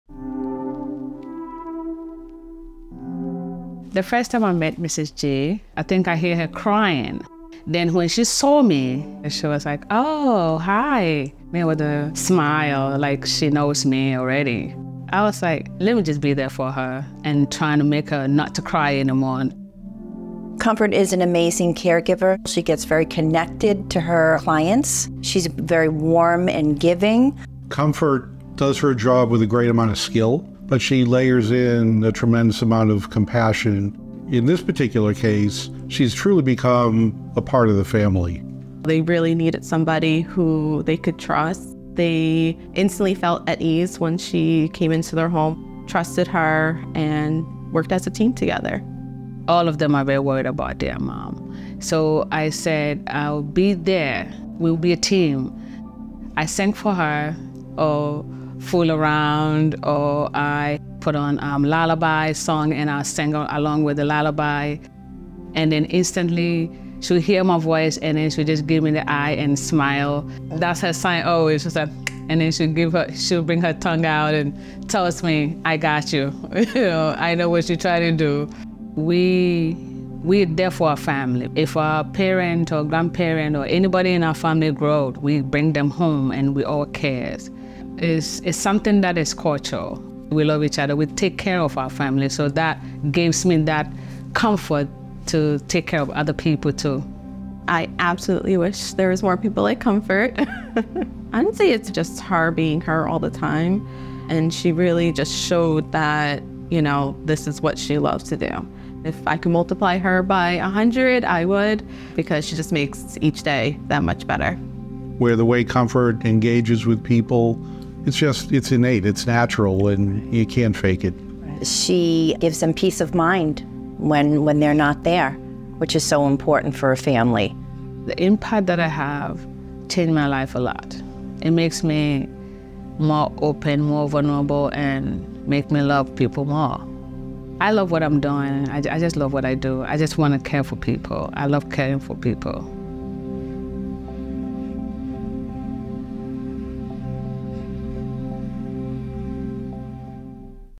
Compassionate Memory Care with Team Approach | BrightStar Care® of Northern Middlesex, NJ
Caregiver Comfort Amponsah lives up to her name – in countless ways. Nominated by the adult children of a 94-year-old client in declining health, Comfort provided home care offering physical, mental and emotional support to their mother who was bed-bound with advanced dementia and other health challenges.

“As a daughter and a nurse, I’ve been intimately involved in the care of my mom over the past several years and it can be hard for me ‘to let go’ and let others do the caring,” said Amy G., who nominated Comfort for Caregiver of the Year. “The minute I met Comfort, my concerns and anxiety were immediately allayed. ‘We’ll work as a team to do the best for your mom’ was the first thing Comfort said when we met.” They connected when Comfort became a live-in caregiver to the family through BrightStar Care® in Northern Middlesex, New Jersey.

Comfort knows that one major aspect of dementia care is that behavior can change quickly from calm to agitated to frightened or tearful. She knows how to adapt to all these situations and often anticipates a change so she can intervene to minimize the impact on her client by dancing and singing to Mrs. G.

Among her siblings, Amy lives the closest to her mom, so much of the personal care has fallen onto her shoulders. She explains that being her mother’s caregiver became her first and foremost role, while her role as daughter was slipping further down.

Comfort made it possible for Amy and her siblings to resume their roles as loving family members while Comfort met their mother’s extensive needs as her attentive and intuitive caregiver – which is the ultimate goal of in-home memory care. Watch the story of how Comfort became a godsend to the family.